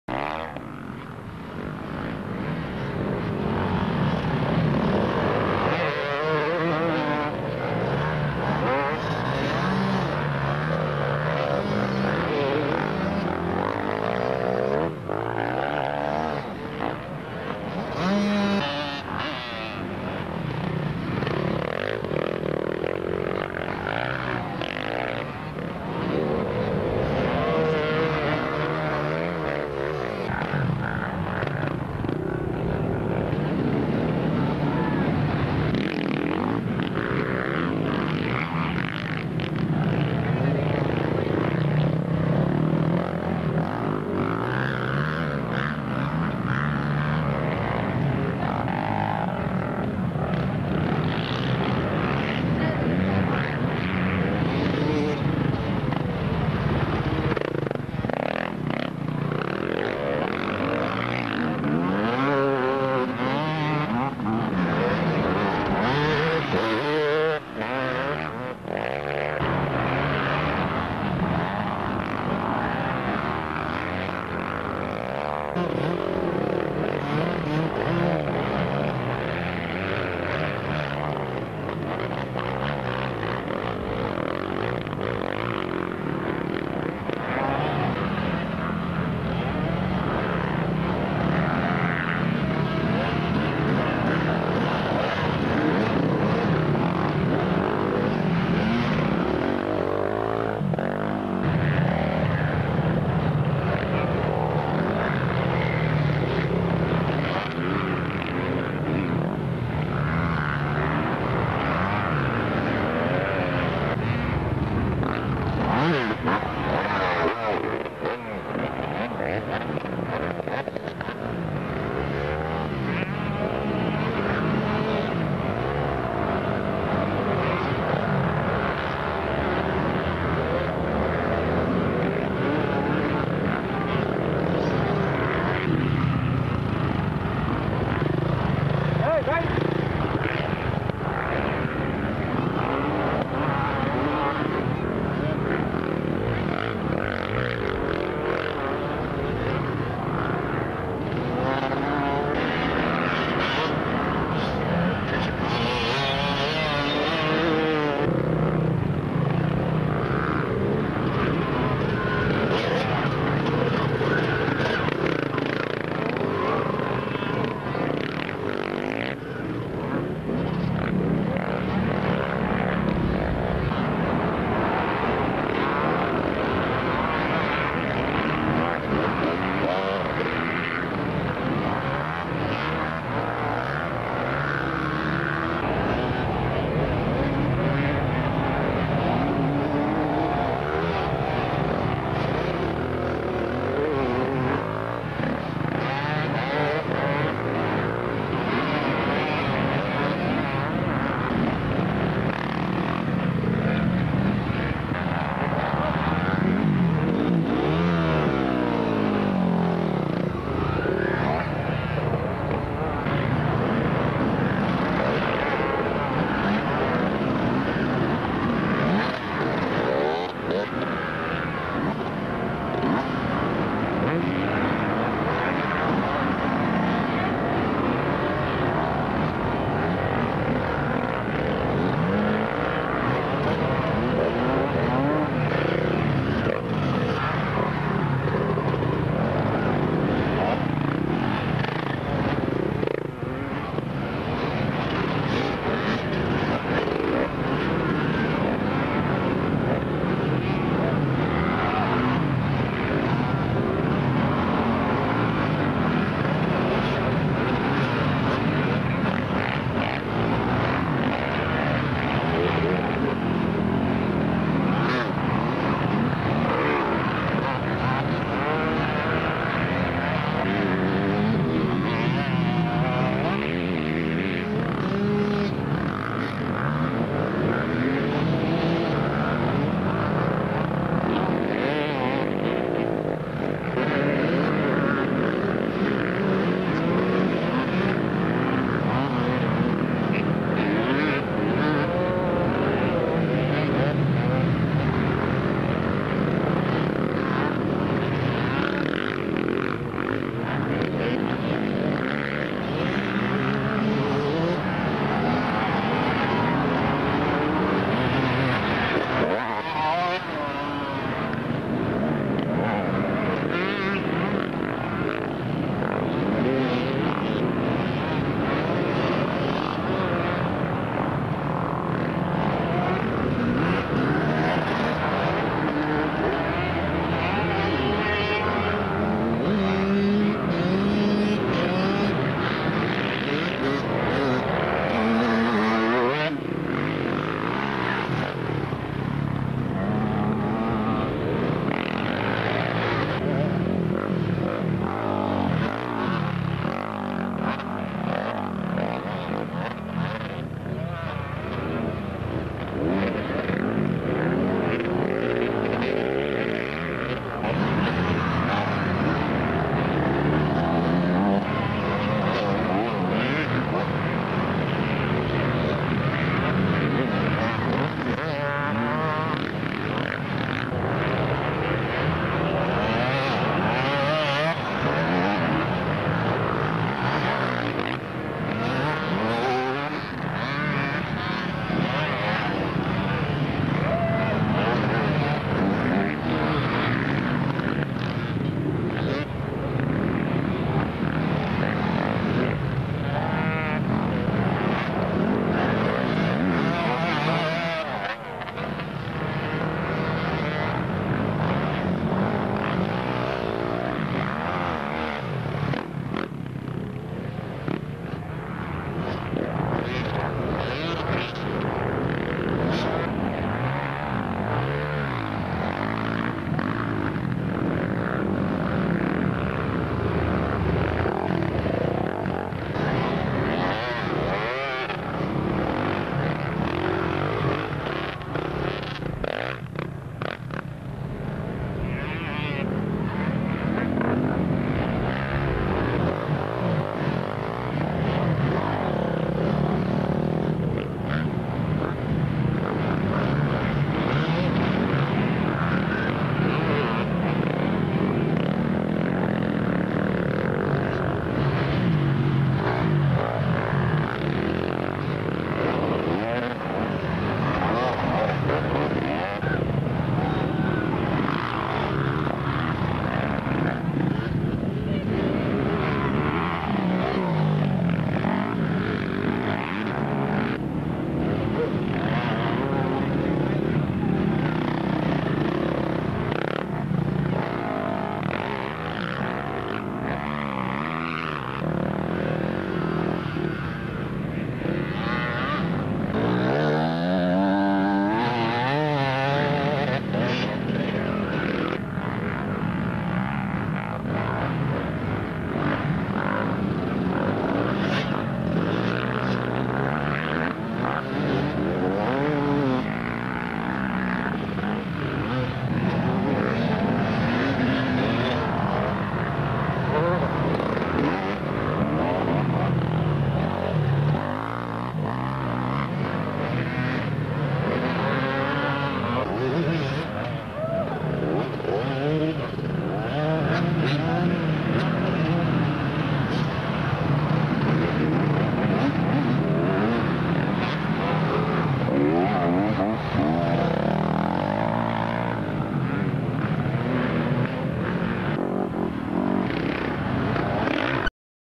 Isle of Man Boxing day 2 man enduro 2004 pt 2
Billy and Jimmy 56, Owl and Gary 94, Sid and Shod, Team Gink, 69, Dave Coz 5, Al Walkie Stewart 11, Dion the ragged man 37